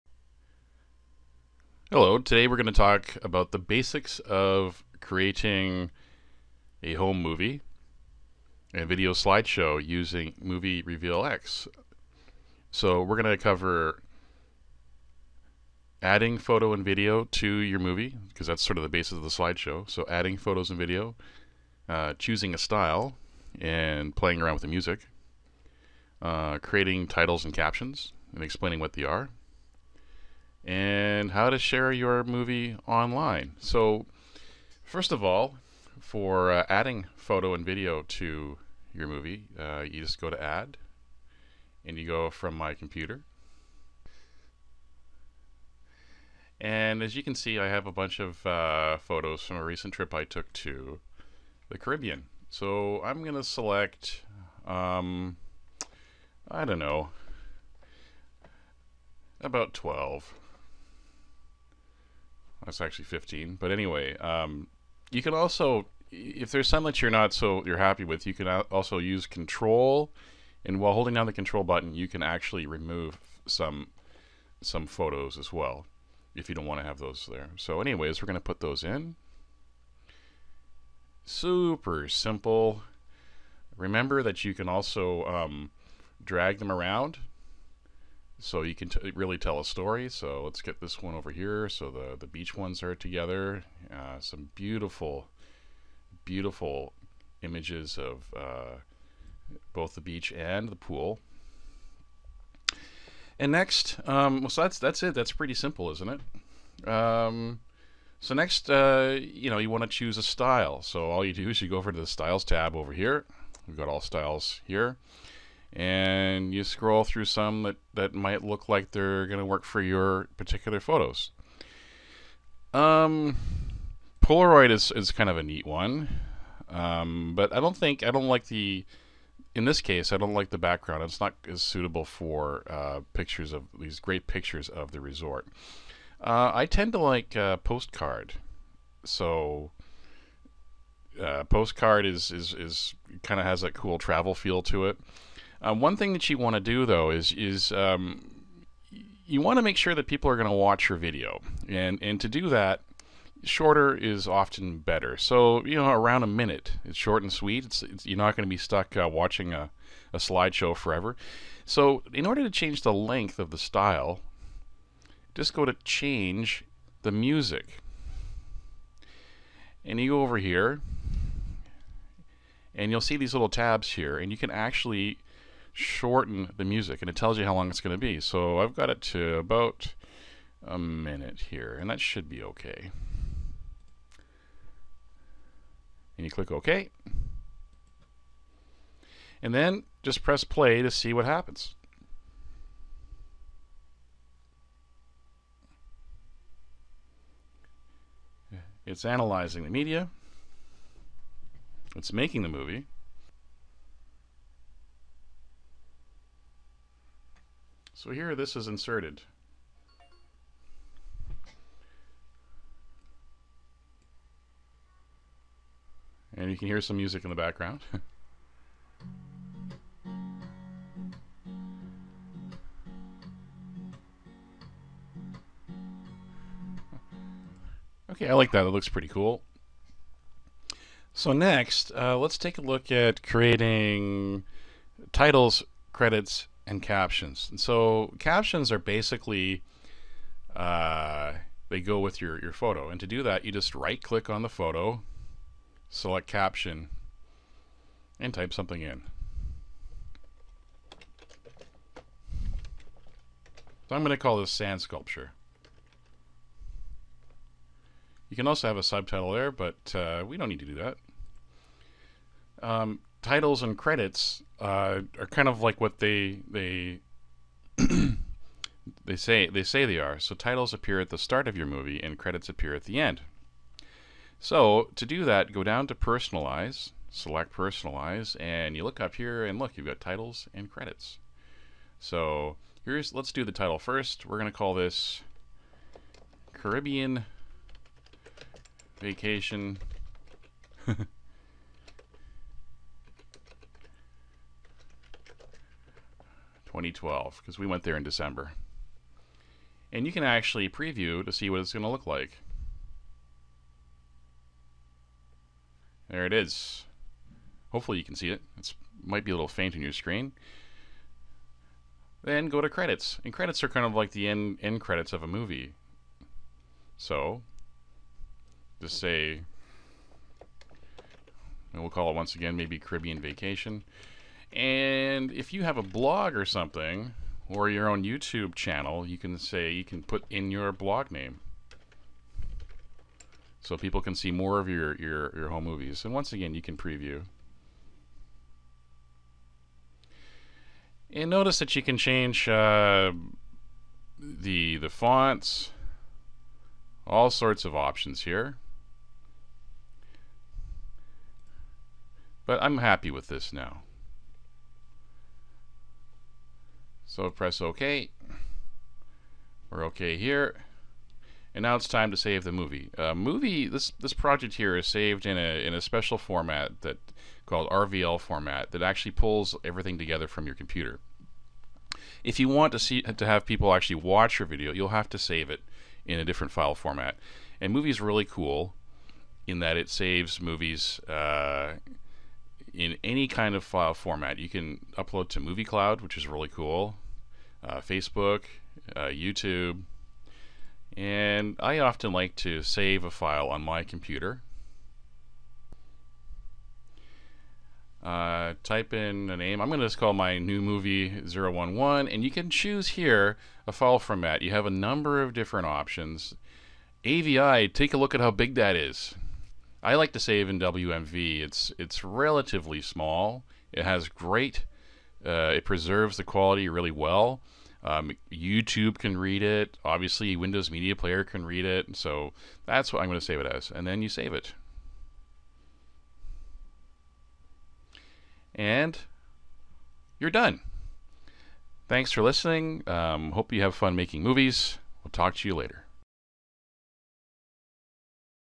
Basic muvee Reveal photo slideshow software tutorial
Learn how to use our slideshow maker to add quickly add photos. We've also included some Windows tips and tricks for selecting a range of photos

Load muvee up, and select "new project" under File, in the top left hand corner of the window.

To add your photos, click on the "Add" button in the photos and video tab.

There are a number of different options to choose from to add photos - you can add photos and movies from everything from your computer to camcorders.

Select your photos.  muvee Reveal X makes it possible to select and add multiple photos at the same time.

There are two ways to do this:

1) Use the "SHIFT" button to select a range of photos

2) If you only want to choose just a few photos, try using the "CTRL" button. Using CTRL and clicking on a photo allows you to select just the photos that you want.

You now have a whole bunch of photos that you can play with to make a slideshow.

How to choose Styles

Choose the Style that works best with your photos

Do this by selecting the Style tab and scrolling through the various selections.

New Styles are easy to get - there are dozens to choose from, and all help set the tone and mood for your video.

Find a Style you like, and don't be afraid to experiment. The best thing to do is to just try out different Styles.

Try out Styles by selecting the Style and hitting play - you will need to have added photos first.

How to create titles, credits and captions with our video slideshow maker
Titles and credits appear before and after your muvee, just like a real muvee
Captions appear with individual photos

Insert a caption by right-clicking on a photo
Titles appear at the beginning of your muvee, and credits appear at the end, just like in a real movie! You can put your name there, where you went, or even your blog URL - anything at all.

Select  "Title" in the top left corner.

You'll be presented with a number of options, including different fonts, font sizes, and you'll even have the ability to choose a background image for your title.

How to make credits
To personalize credits, select  "Credits" from the Personlize menu, just as you did with titles.

How to save your muvee
muvee Reveal X project files are stored in RVL format - this format is only used by muvee Reveal X.

So, you'll have to save your muvee in a file format that can be viewed on a PC, Mac computer, smartphone or on Facebook and YouTube.

Click the "Save muvee" button. After that, it's really simple.